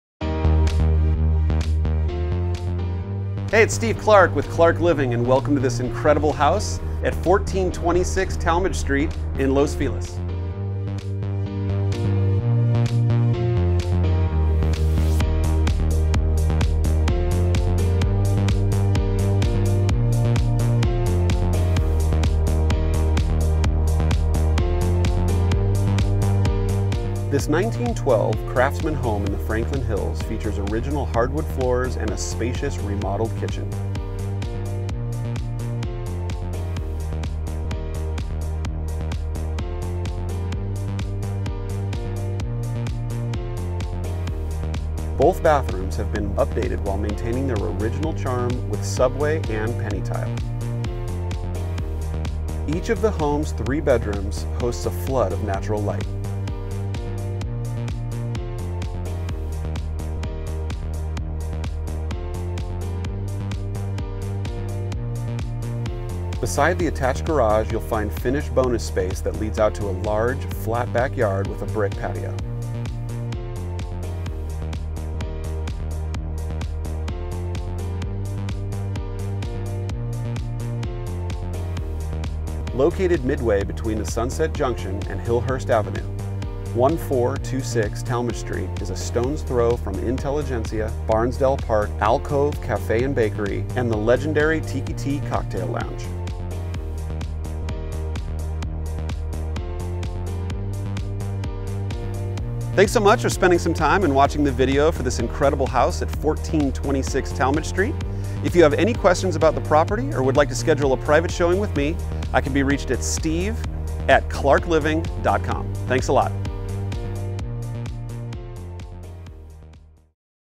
1426 Talmadge | 1912 Los Feliz Craftsman
This 1912 Craftsman home in Los Feliz’s Franklin Hills features original hardwood floors and a spacious remodeled kitchen. Both bathrooms have been updated while maintaining their original charm with subway and penny tile. Each of the home’s three bedrooms host a flood of natural light. Beside the attached garage you’ll find finished bonus space that leads out to a large, flat backyard with a brick patio. Located midway between Sunset Junction and Hillhurst Avenue, 1426 Talmadge St is a stone’s throw from Sunset Junction, Barnsdall Park, and the legendary Tiki Ti Cocktail Lounge.